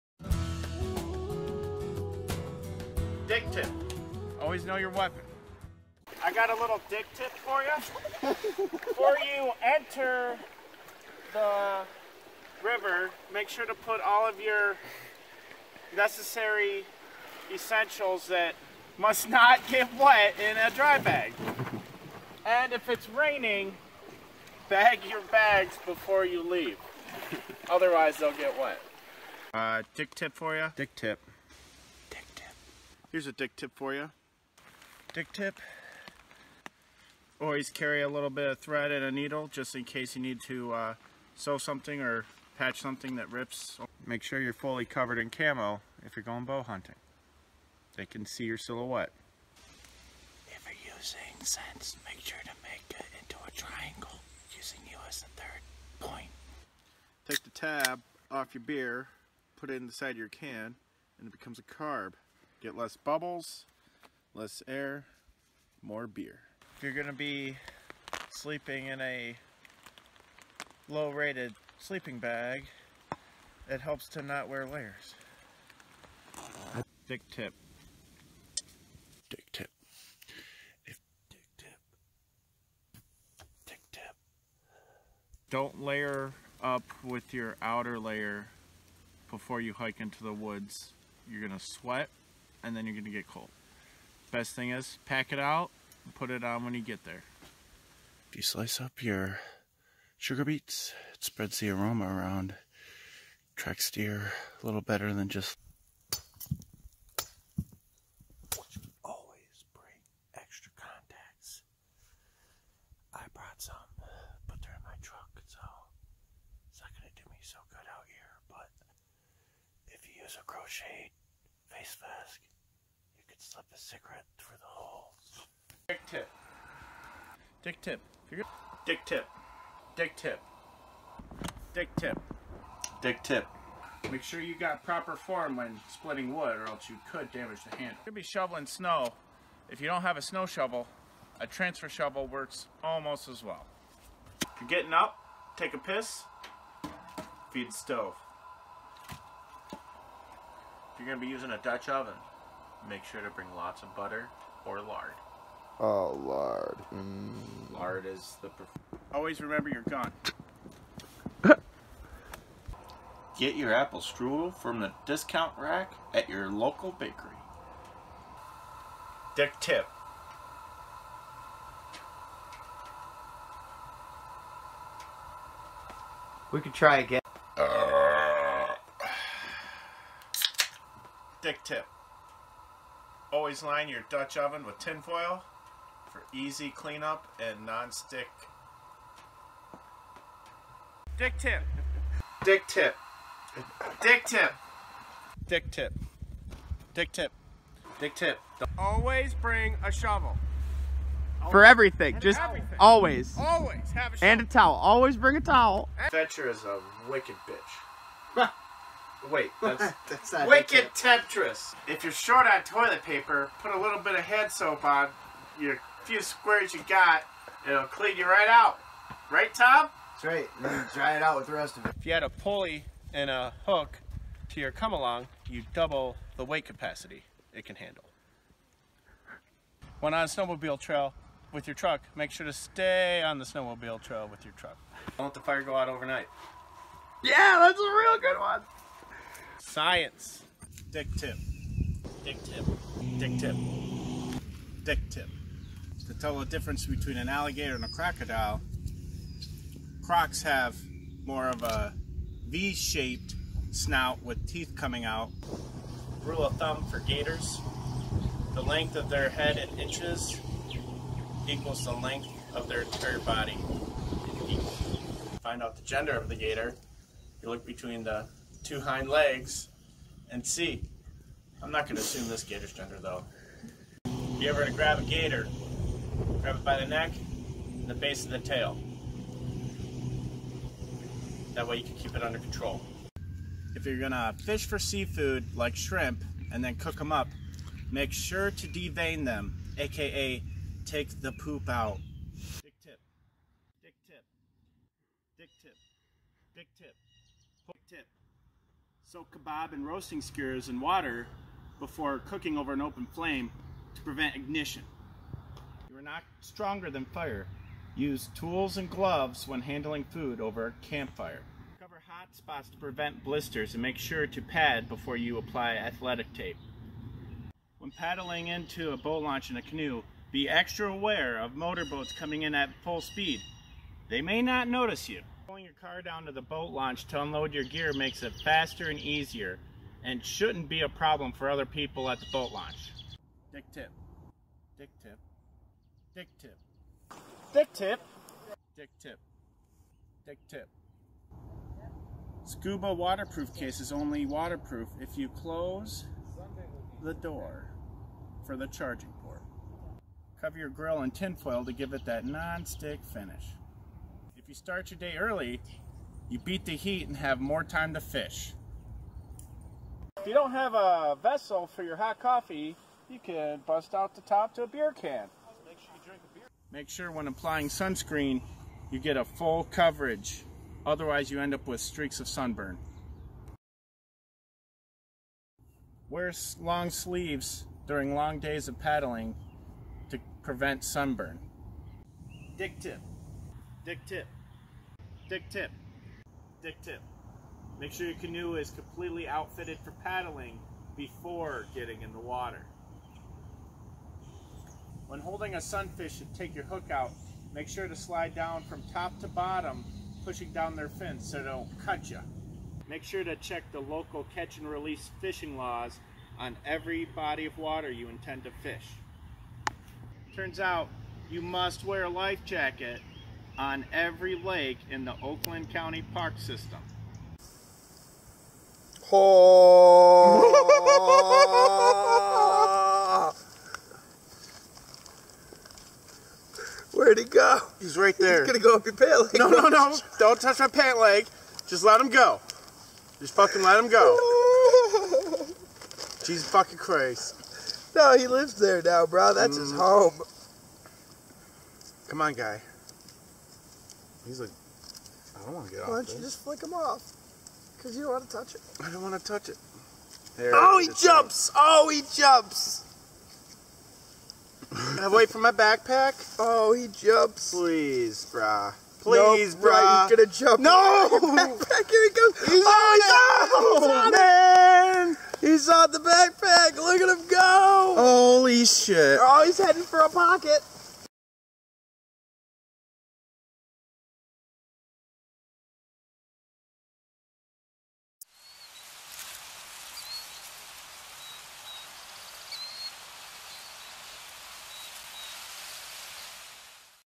DickTip Season One Compilation...
As we wrap up Season One of adventuring, we thought we would take a look back on the year with the only thing our fans actually care about... Loads of DickTips at the ready.
But don't worry, Season Two is already in the works... for those of you that watch every adventure. Thank you. Both of you.

Logo: @BIT_Spookery

Opening/Closing Graphics: @This_Show